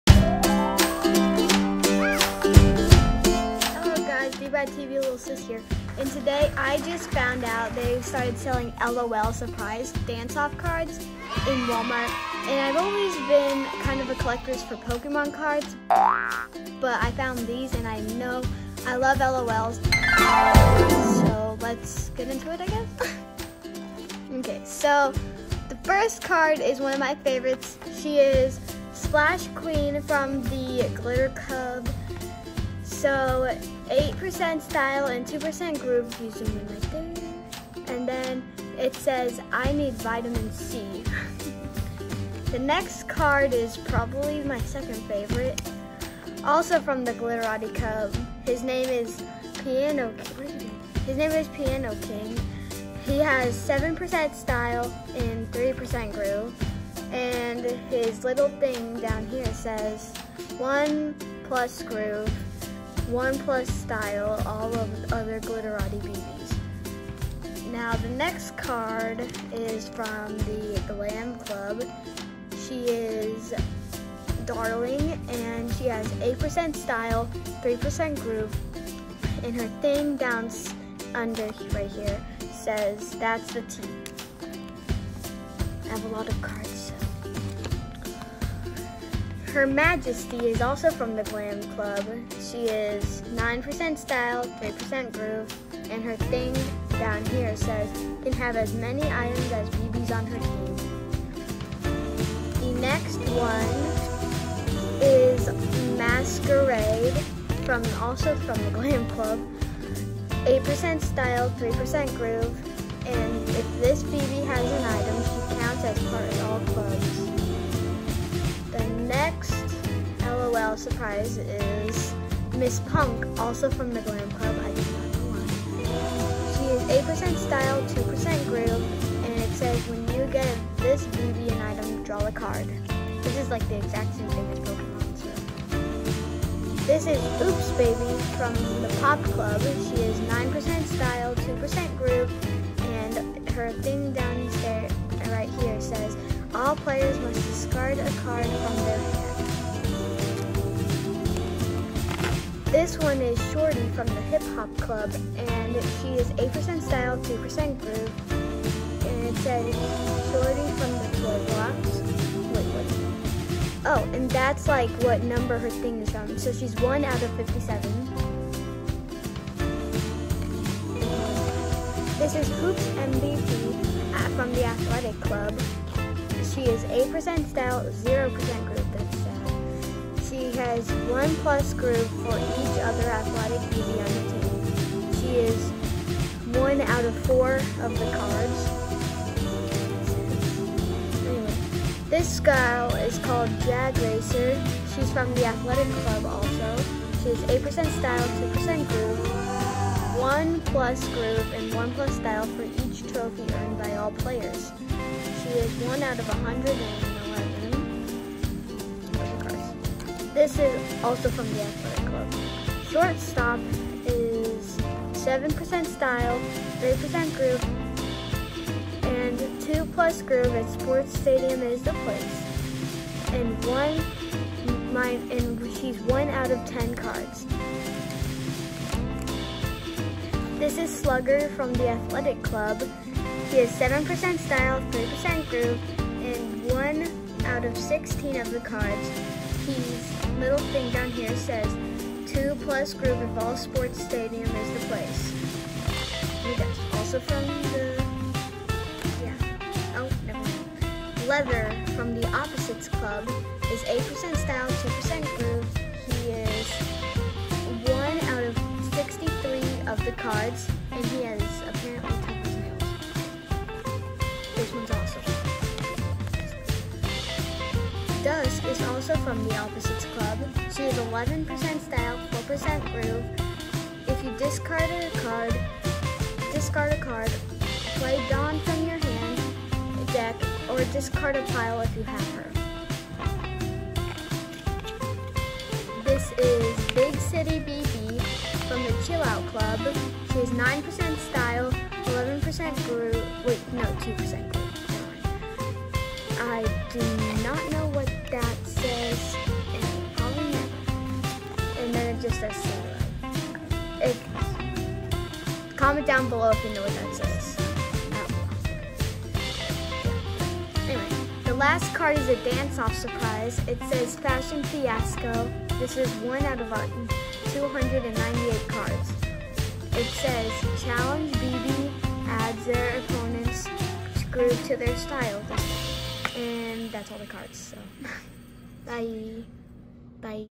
LOL Surprise Dance Off Cards!
Saw these at the store and decided to get some! I love LOL Surprise dolls so I thought these would be cool!